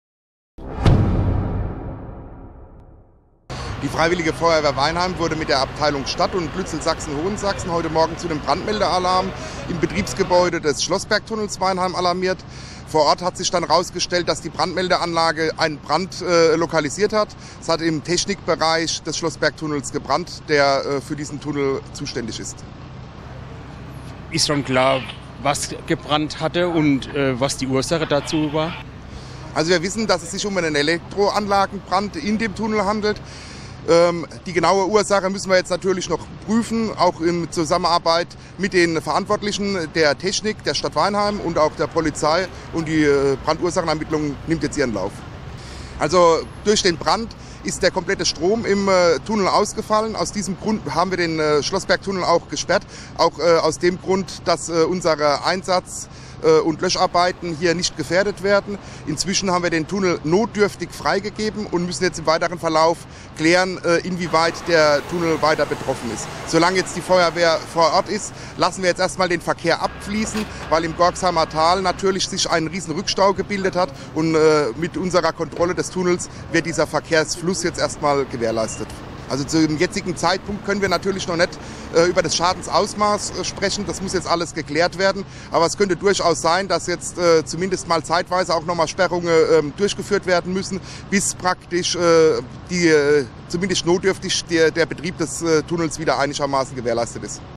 Brand in Technikzentrale des Schlossbergtunnels - komplette Technik im Tunnel ausgefallen
Erstmeldung Polizei: "Auf Grund eines Brands in einem Schaltkasten der Technikzentrale des Schlossbergtunnels ist dieser aktuell gesperrt. Kräfte der Feuerwehr und der Polizei sind im Einsatz. Der Verkehr wird umgeleitet." Quelle: Polizeipräsidium Mannheim

Zu einem Brand ist es am Donnerstagmorgen im Weinheimer Schlossbergtunnel gekommen. Nach ersten Informationen wurde der Brandmeldealarm ausgelöst. Beim Eintreffen der Feuerwehreinsatzkräfte brannte im Betriebsraum die Elektroanlage. Diese konnte schnell gelöscht werden. Durch das Feuer ist die komplette Technik im Tunnel ausgefallen. Der Tunnel wurde erst gesperrt. Inzwischen ist er aber vorübergehend wieder geöffnet. Ralf Mittelbach von der Feuerwehr Weinheim: "Solange die Feuerwehr vor Ort ist, lassen wir den Verkehr abfließen". Es kam durch die Tunnelsperrung zu erheblichen Verkehrsbehinderungen. Im Gorxheimer Tal hat sich ein riesiger Rückstau gebildet. Die Brandursache ist derzeit noch unklar. Die Ermittlungen wurden aufgenommen.